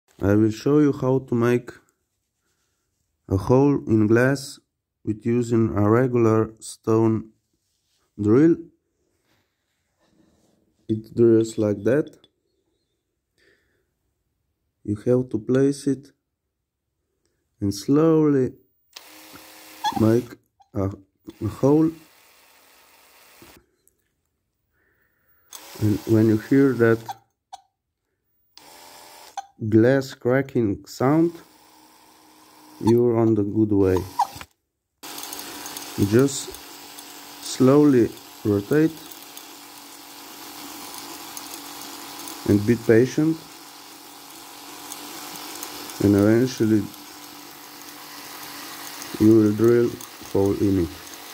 drilling in glass bottle
Its hard to drill in glass but you can do it with patience and regular Stone drill bits and cordless drill.hope i give you an idea how to make it..i use regular beer bottle but you can drill in anything.